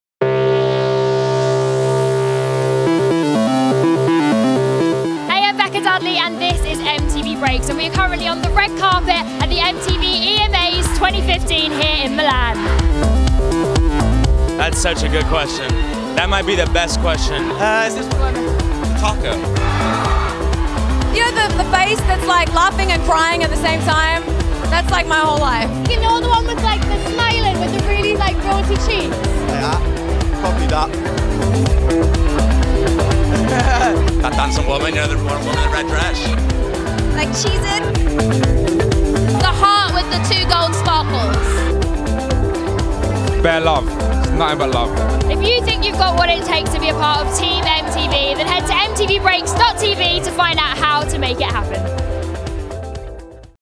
Celebrity Careers In One Emoji | EMA 2015 Red Carpet
We asked a host of stars on the EMA 2015 red carpet in Milan including Macklemore, Rudimental, Charlie XCX and Martin Garrix to describe their careers in one emoji!